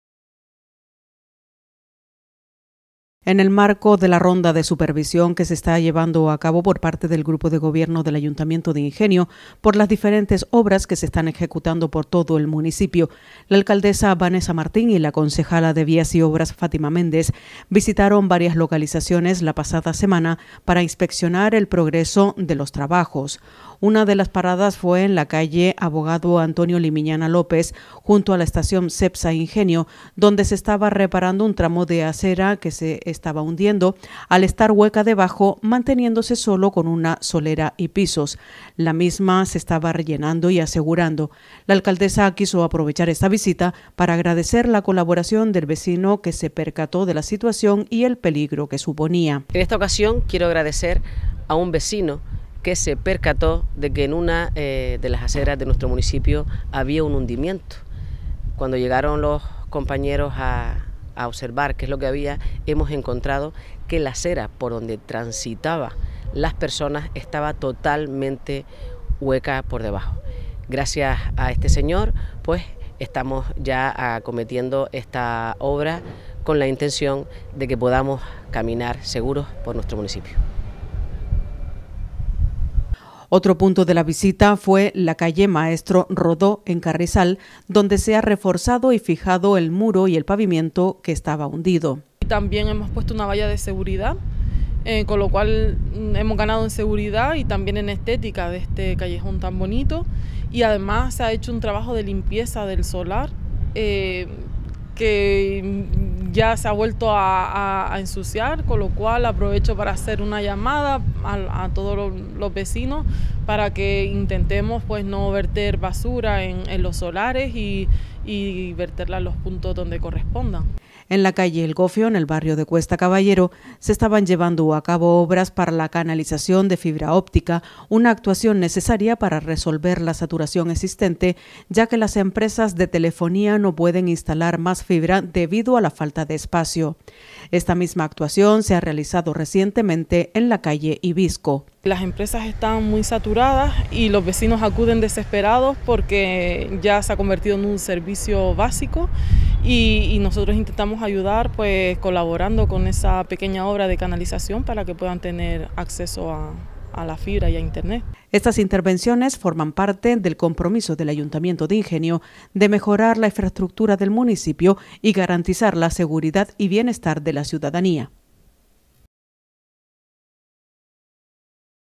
Alcaldesa y Concejala de Vías y Obras supervisan actuaciones para mejorar seguridad y conectividad
La alcaldesa y la concejala de Vías y Obras supervisan actuaciones para mejorar en seguridad y conectividad

Recorrieron las calles Abogado Antonio Limiñana López y Maestro Rodó donde se han reparado hundimientos y una obra de canalización de fibra en El Gofio

En el marco de la ronda de supervisión que está llevando a cabo el grupo de gobierno del Ayuntamiento de Ingenio por las diferentes obras que se están ejecutando por todo el municipio, la alcaldesa Vanesa Martín y la concejala de Vías y Obras, Fátima Méndez, visitaron varias localizaciones la pasada semana para inspeccionar el progreso de los trabajos.

Una de las paradas fue en la Calle Abogado Antonio Limiñana López, junto a la estación CEPSA Ingenio, donde se estaba reparando un tramo de acera que se estaba hundiendo, al estar hueca debajo, manteniéndose solo con una solera y pisos. La misma se estaba rellenando y asegurando. La alcaldesa quiso aprovechar esta visita para agradecer la colaboración del vecino que se percató de la situación y el peligro que ello suponía.

Otro punto de la visita fue la Calle Maestro Rodó en Carrizal, donde se ha reforzado y fijado el muro y el pavimento que estaba hundido. "También hemos puesto una nueva valla, ganando en seguridad y en estética en este callejón," detalló Fátima Méndez. Además, se realizó un trabajo de limpieza de todo el solar, aunque lamentablemente ya se ha vuelto a ensuciar. "Aprovecho para hacer un llamado a la población para que no viertan basura en los solares," añadió.

En la Calle El Gofio en el barrio de Cuesta Caballero, se estaban llevando a cabo obras para la canalización de fibra óptica, una actuación necesaria para resolver la saturación existente, ya que las empresas de telefonía no pueden instalar más fibra debido a la falta de espacio. Esta misma actuación se ha realizado recientemente en la Calle Hibisco. "Las empresas están saturadas y los vecinos acuden desesperados a nosotros porque la fibra se ha convertido en un servicio básico," comentó Méndez. "Nosotros intentamos ayudar colaborando con estas obras de canalización para que puedan tener acceso a Internet."

Estas intervenciones forman parte del compromiso del Ayuntamiento de Ingenio de mejorar la infraestructura del municipio y garantizar la seguridad y bienestar de la ciudadanía.